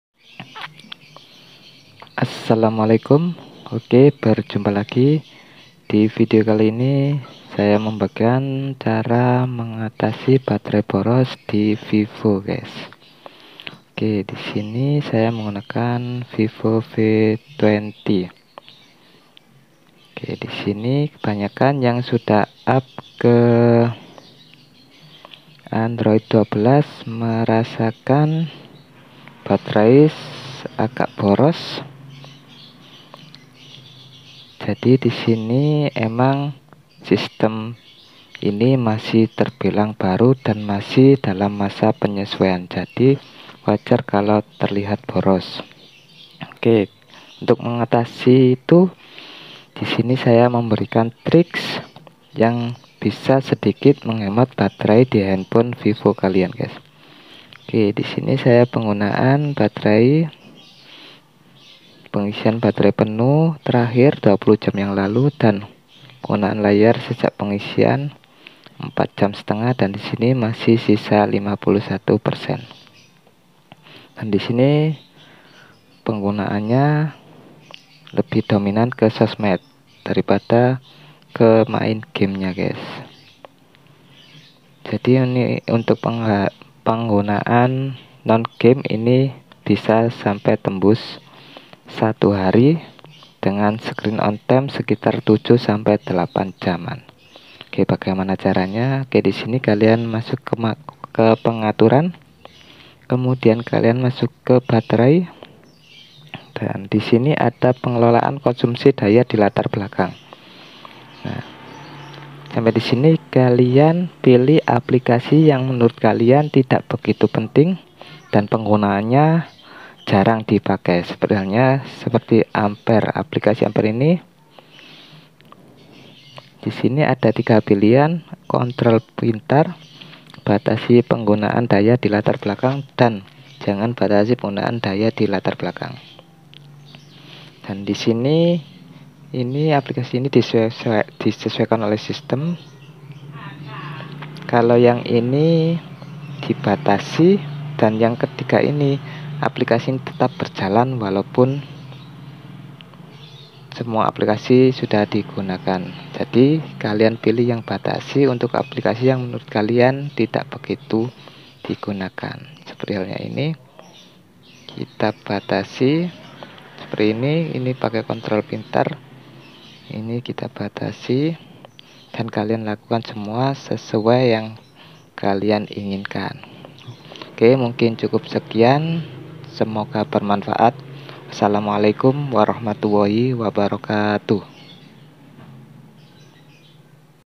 Cara Mengatasi Baterai Boros Pada Handphone Vivo | Tested V21 4G
Di video kali ini saya mau makan cara mengatasi baterai boros di handphone Vivo di sini saya menggunakan HP Vivo v21 4G, pada settingan default HP ini memiliki aplikasi hampir semuanya berjalan di latar belakang jadi di sini kita akan membatasi aplikasi yang sekiranya tidak kita perlukan supaya tidak berjalan di latar belakang nah bagaimana cara membatasi aplikasi tersebut oke simak video berikut